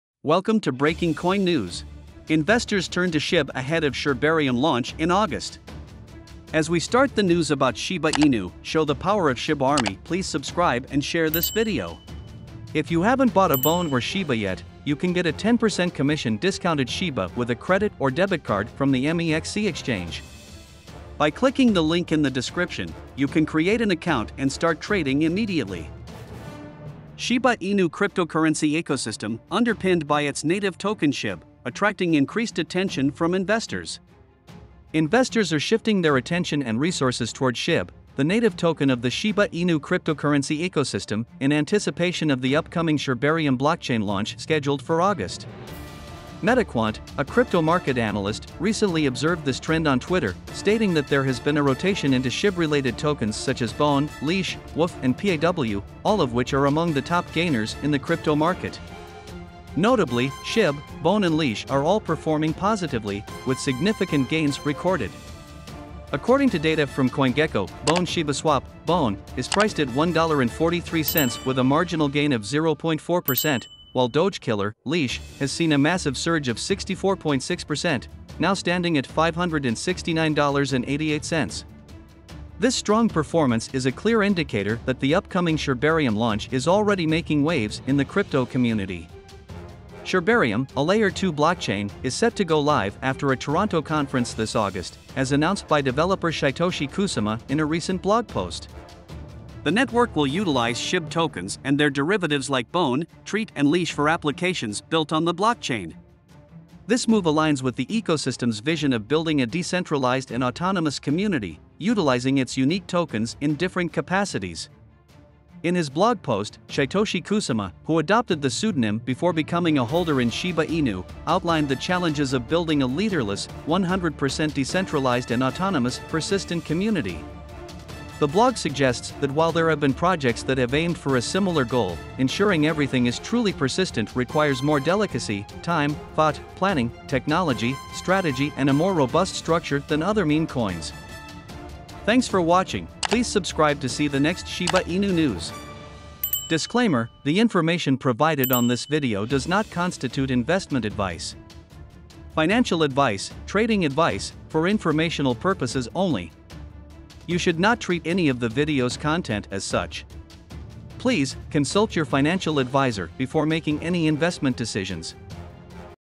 Breaking Coin | Investors Turn to SHIB Ahead of Shibarium Launch in August
Investors Turn to SHIB Ahead of Shibarium Launch in August
Shiba Inu cryptocurrency ecosystem, underpinned by its native token SHIB, attracting increased attention from investors

to read the original text on U.Today ( News, Price Analysis, Trading Guide, Crypto )
Investors are shifting their attention and resources toward SHIB, the native token of the Shiba Inu cryptocurrency ecosystem, in anticipation of the upcoming Shibarium blockchain launch scheduled for August.
Metaquant, a crypto market analyst, recently observed this trend on Twitter, stating that there has been a rotation into SHIB-related tokens such as BONE, LEASH, WOOF and PAW, all of which are among the top gainers in the crypto market.  Notably, SHIB, BONE and LEASH are all performing positively, with significant gains recorded. According to data from CoinGecko, Bone ShibaSwap (BONE) is priced at $1.43 with a marginal gain of 0.4%, while Doge Killer (LEASH) has seen a massive surge of 64.6%, now standing at $569.88. This strong performance is a clear indicator that the upcoming Shibarium launch is already making waves in the crypto community. Shibarium, a Layer 2 blockchain, is set to go live after a Toronto conference this August, as announced by developer Shytoshi Kusama in a recent blog post.
The network will utilize SHIB tokens and their derivatives like BONE, TREAT and LEASH for applications built on the blockchain. This move aligns with the ecosystem's vision of building a decentralized and autonomous community, utilizing its unique tokens in different capacities. In his blog post, Shytoshi Kusama, who adopted the pseudonym before becoming a holder in Shiba Inu, outlined the challenges of building a "leaderless, 100% decentralized and autonomous persistent community." The blog suggests that while there have been projects that have aimed for a similar goal, ensuring everything is truly "persistent" requires more delicacy, time, thought, planning, technology, strategy and a more robust structure than other meme coins.